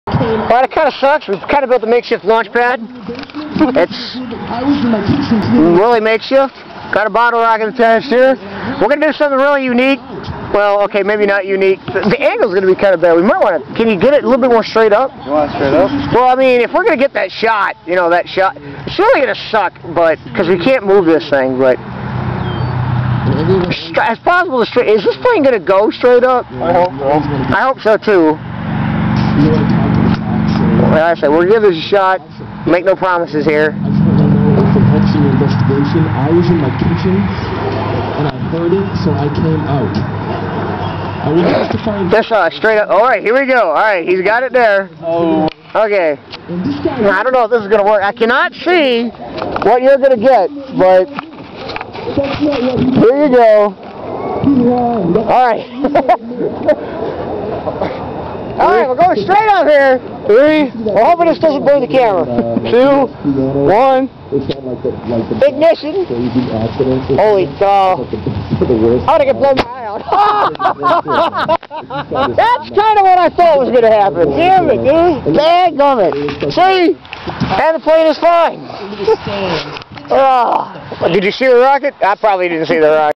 Missile/Rocket Launch at Knoll IS 1
The first rocket launch at Knoll, as we try to see how high a KIA aircraft can be taken via bottle rocket. We offer a unique perspective from the makeshift pad in an attempt to follow the launch as far as possible.